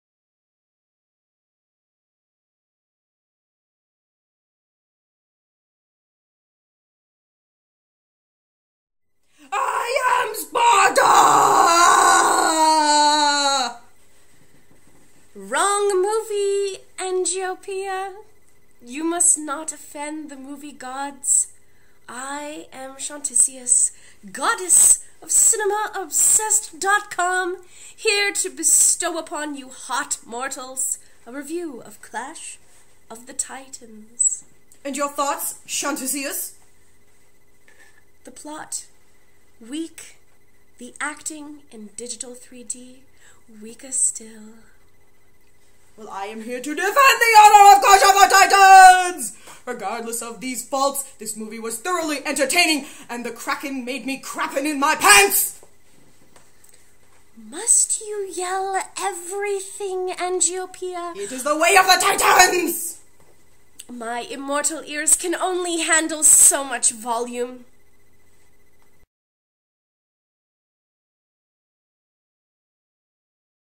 The 89 Second Review: Clash of the Titans
Chantisius and Angiopoeia of CinemaObsessed.com review the new Clash of the Titans for the New Hot 89.9!